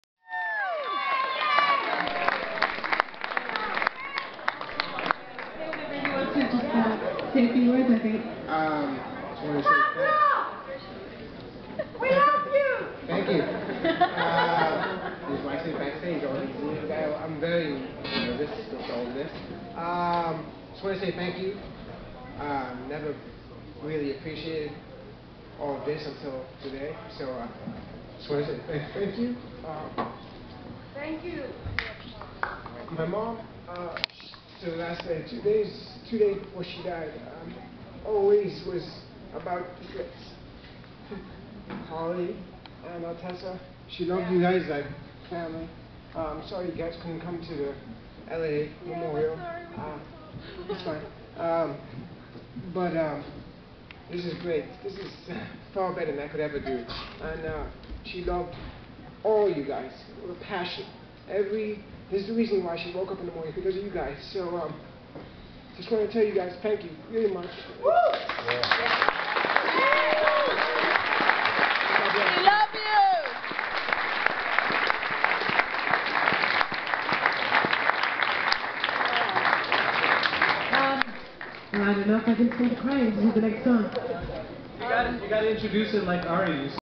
Ari Up Memorial - ari's son Pablo
A very moving speech by Ari's son Pablo (there is also twin Pedro & younger son Wilton)  which reduced everyone to tears.